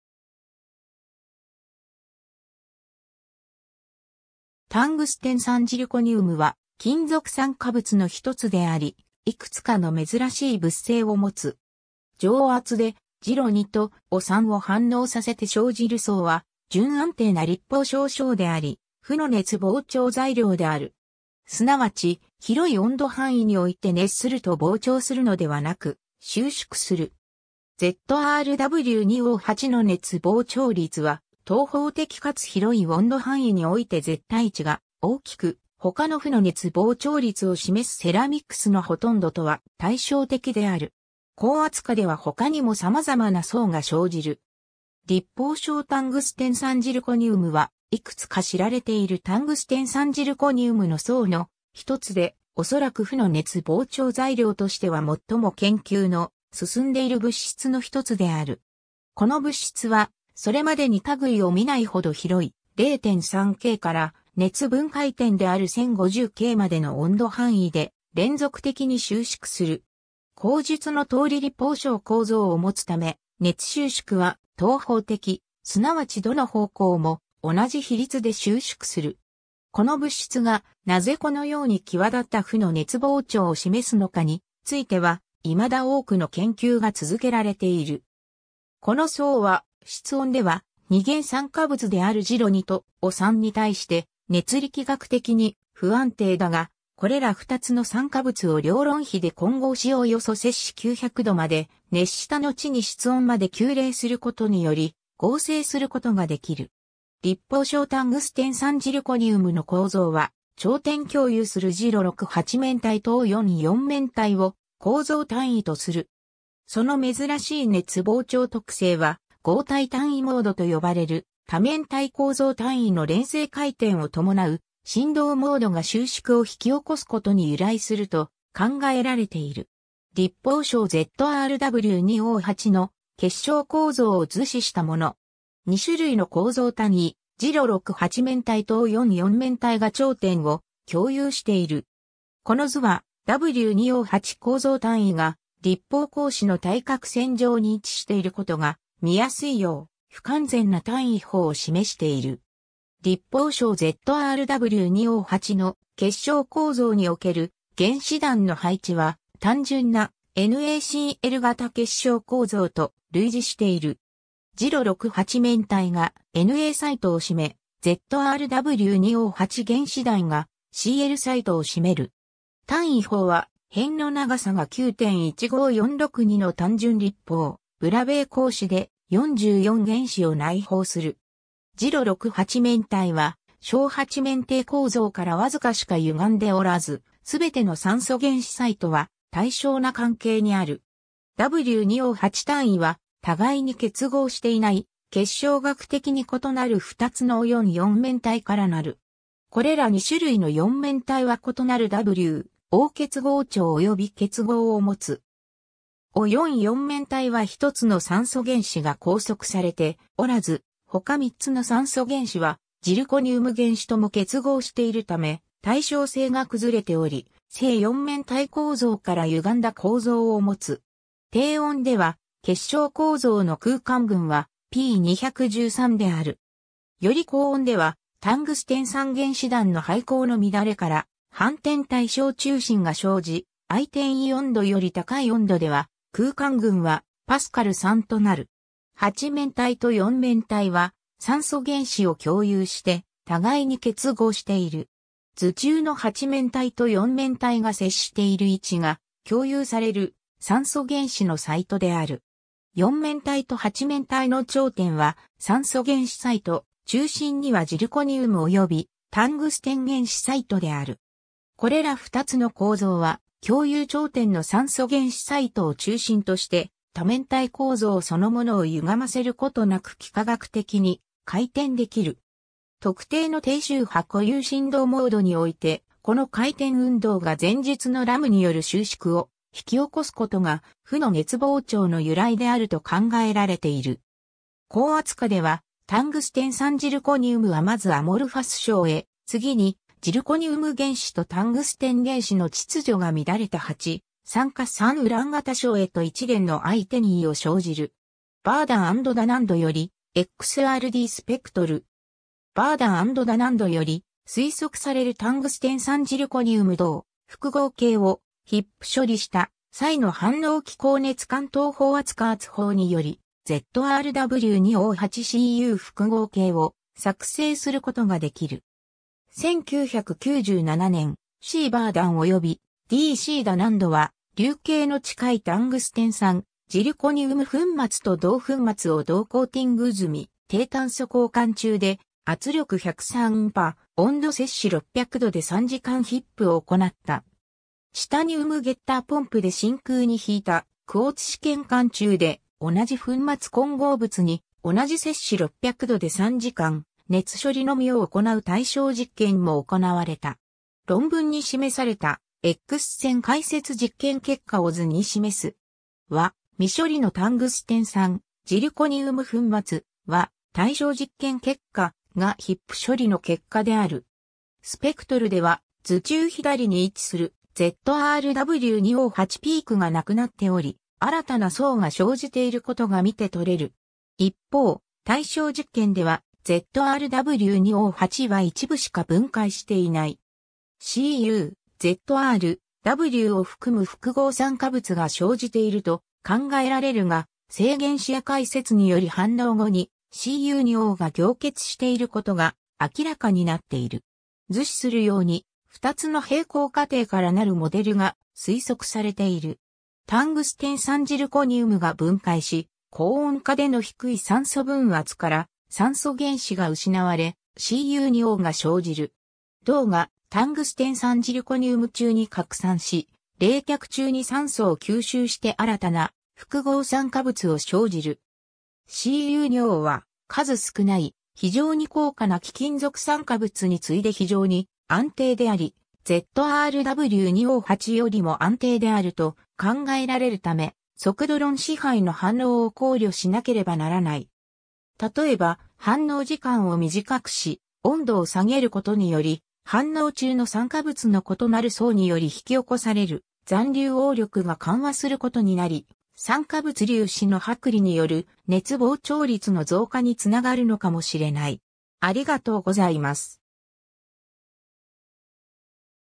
タングステン酸ジルコニウム
タングステン酸ジルコニウム（タングステンさんジルコニウム、英: Zirconium Tungstate, Zr(WO4)2, ZrW2O8）は、金属酸化物の一つであり、いくつかの珍しい物性をもつ。
常圧で ZrO2 と WO3 を反応させて生じる相は準安定な立方晶相であり、「負の熱膨張材料」である。
すなわち、広い温度範囲において熱すると膨張するのではなく収縮する。
ZrW2O8 の熱膨張率は等方的かつ広い温度範囲 (−273〜777 °C) において絶対値が大きく（平均膨張率 −7.2×10−6 K−1）、他の負の熱膨張率を示すセラミックスのほとんどとは対照的である。
高圧下では他にも様々な相が生じる。
 立方晶タングステン酸ジルコニウム (α-ZrW2O8) は、いくつか知られているタングステン酸ジルコニウムの相の一つで、おそらく負の熱膨張材料としては最も研究の進んでいる物質の一つである。
この物質は、それまでに類を見ないほど広い、 0.3 K から熱分解点である 1050 K までの温度範囲で連続的に収縮する。
後述の通り立方晶構造をもつため、熱収縮は等方的、すなわちどの方向も同じ比率で収縮する。
この物質がなぜこのように際立った負の熱膨張を示すのかについては、未だ多くの研究が続けられている。
 この相は室温では、二元酸化物である ZrO2 と WO3 に対して熱力学的に不安定だが、これら二つの酸化物を量論比で混合しおよそ 900 °C まで熱したのちに室温まで急冷することにより合成することができる。
 立方晶タングステン酸ジルコニウムの構造は頂点共有する ZrO6 八面体と WO4 四面体を構造単位とする。
その珍しい熱膨張特性は、剛体単位モード (RUM) と呼ばれる、多面体構造単位の連成回転を伴う振動モードが収縮を引き起こすことに由来すると考えられている。
 立方晶 ZrW2O8 の結晶構造を図示したもの。
2種類の構造単位、ZrO6 八面体（緑）と WO4 四面体（赤）が頂点を共有している。
この図は、W2O8 構造単位が立方格子の対角線上に位置していることが見やすいよう、不完全な単位胞を示している。
 立方晶 ZrW2O8 の結晶構造における原子団の配置は、単純な NaCl 型結晶構造と類似している。
ZrO6 八面体が Na サイトを占め、 ZrW2O8 原子団が Cl サイトを占める。
単位胞は辺の長さが 9.15462 Å の単純立方ブラベー格子で、44原子を内包する。
 ZrO6 八面体は正八面体構造からわずかしか歪んでおらず、全ての酸素原子サイトは対称な関係にある。
W2O8 単位は、互いに結合していない結晶学的に異なる2つの WO4 四面体からなる。
これら2種類の四面体は異なる W-O 結合長および結合を持つ。
WO4 四面体は1つの酸素原子が拘束されておらず（中心タングステン (W) 原子にしか結合していない）、他3つの酸素原子はジルコニウム原子とも結合している（すなわち頂点共有されている）ため、対称性が崩れており正四面体構造から歪んだ構造を持つ。
 低温では結晶構造の空間群は P213 である。
より高温ではタングステン酸原子団の配向の乱れから反転対称中心が生じ、相転移温度 (~180 °C) より高い温度では空間群は Pa3 となる。
 八面体と四面体は酸素原子を共有して互いに結合している。
図中の八面体と四面体が接している位置が共有される酸素原子のサイトである。
四面体と八面体の頂点は酸素原子サイト、中心にはジルコニウムおよびタングステン原子サイトである。
これら二つの構造は、共有頂点の酸素原子サイトを中心として、多面体構造そのものを歪ませることなく幾何学的に回転できる。
特定の低周波固有振動モードにおいて、この回転運動が前述の RUM による収縮を引き起こすことが負の熱膨張の由来であると考えられている。
 高圧下では、タングステン酸ジルコニウムはまずアモルファス相へ、次にジルコニウム原子とタングステン原子の秩序が乱れた八酸化三ウラン型相へと一連の相転移を生じる。
 Verdon & Dunand (1997) より、XRD スペクトル。
 Verdon & Dunand (1997) より、推測されるタングステン酸ジルコニウム-銅複合系を HIP 処理した際の反応機構 熱間等方圧加圧法 (HIP) により、ZrW2O8-Cu 複合系を作成することができる...